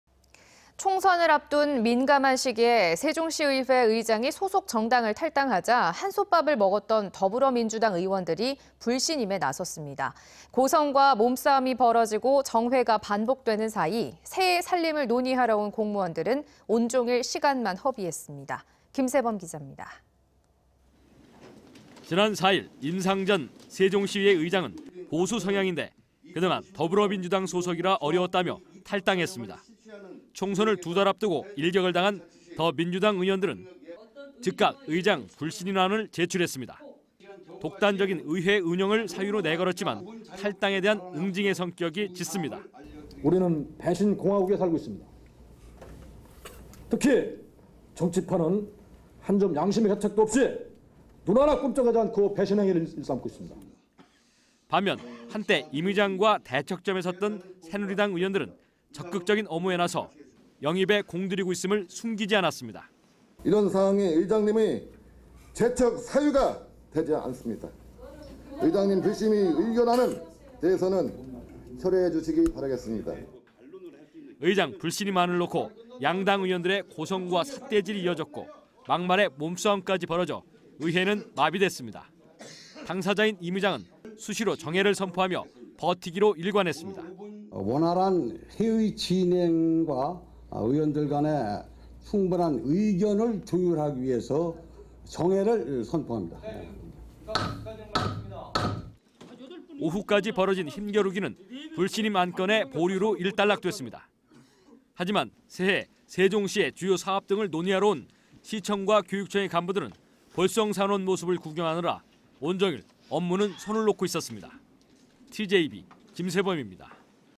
[TJB 8뉴스] 명품도시에…함량미달 의회
【 앵커멘트 】

총선을 앞둔 민감한 시기에
세종시의회 의장이
소속정당을 탈당하자,
한솥밭을 먹었던
더불어민주당 의원들이
불신임에 나섰습니다.

고성과 몸싸움이 벌어지고
정회가 반복되는 사이,
새해 살림을 논의하러 온
공무원들은
온종일 시간만
허비했습니다.

김세범 기잡니다.

【 기자 】

지난 4일 임상전 세종시의회 의장은 보수 성향인데 그동안 더불어민주당 소속이라 어려웠다며
탈당했습니다.

총선을 두 달 앞두고 일격을 당한 더민주 의원들은 즉각 의장 불신임안을 제출했습니다.

독단적인 의회 운영을 사유로 내걸었지만 탈당에 대한 응징의 성격이 짙습니다.

▶ 인터뷰 : 윤형권 / 세종시의회 부의장(더불어민주당)
- "우리는 배신공화국에 살고 있습니다.특히 정치판은 한 점 양심의 가책도 없이 눈 하나 꿈쩍하지 않고 배신행위를 일삼고 있습니다."

반면 한때 임의장과 대척점에 섰던 새누리당 의원들은 적극적인 엄호에 나서 영입에 공들이고 있음을 숨기지 않았습니다.

▶ 인터뷰 : 장승업 / 세종시의회 부의장(새누리당)
- "이런 사안이 의장님의 제척 사유가 되지 않습니다.의장님 불신임 의결안에 대해서는 철회해 주시기 바랍니다."

의장 불신임안을 놓고 양당 의원들의 고성과
삿대질이 이어졌고, 막말에 몸싸움까지
벌어져 의회는 마비됐습니다.

당사자인 임의장은 수시로 정회를 선포하며
버티기로 일관했습니다 .

 ▶ 인터뷰 : 임상전 / 세종시의회 의장
- "원활한 회의진행과 의원들간에 충분한 의견을 조율하기 위해서 정회를 선포합니다."

오후까지 벌어진 힘겨루기는 불신임 안건의
보류로 일단락됐습니다.

하지만, 새해 세종시의 주요 사업 등을 논의하러 온 시청과 교육청의 간부들은 볼썽사나운
모습을 구경하느라 온종일 업무는 손을 놓고
있었습니다.

TJB 김세범입니다.